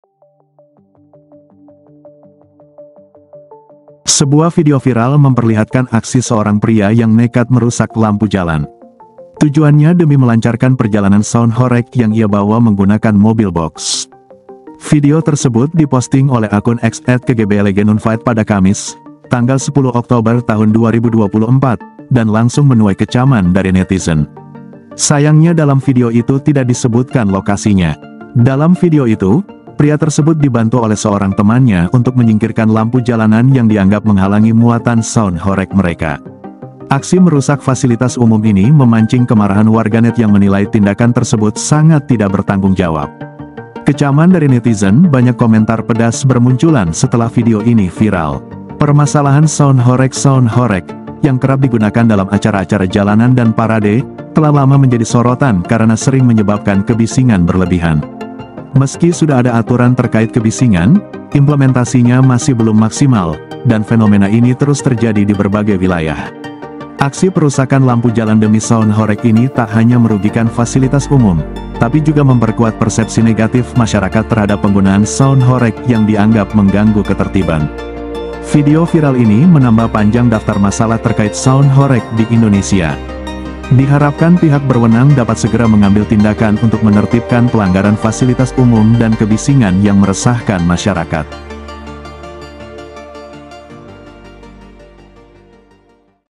Viral, Aksi Kru Sound Horeg Diduga Rusak Lampu Jalan Gegara Truk Tak Bisa Lewat
Sebuah video viral memperlihatkan aksi seorang pria yang nekat merusak lampu jalan. Tujuannya demi melancarkan perjalanan sound horeg yang ia bawa menggunakan mobil boks.

Video tersebut diposting oleh akun X @kegblgnunfaedh pada Kamis (10/10/2024), dan langsung menuai kecaman dari netizen. Sayangnya dalam video itu tidak disebutkan lokasinya.